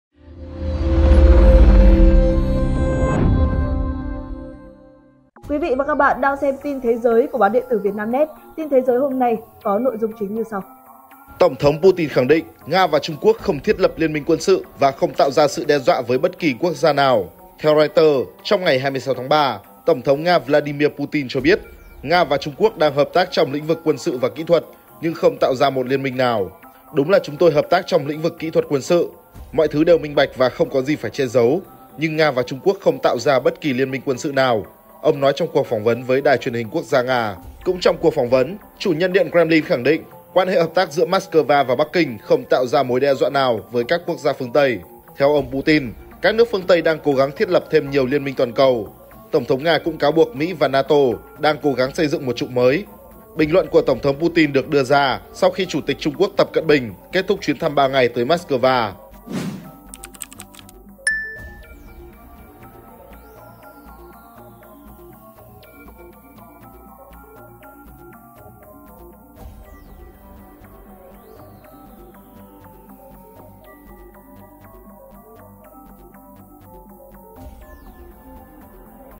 Nga bác tin lập liên minh quân sự với Trung Quốc | Vietnamnet
Tổng thống Putin khẳng định Nga và Trung Quốc không thiết lập liên minh quân sự và không tạo ra sự đe dọa với bất kỳ quốc gia nào.